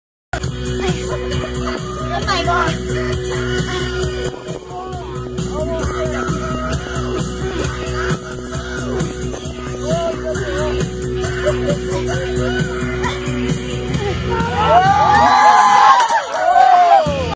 Missy Anderson, Red Wing Minnesota fighting a 55 inch muskie!
She made front cover, in color, of Minnesota Outdoor News 2016 State Fair Edition, their biggest publication of the year.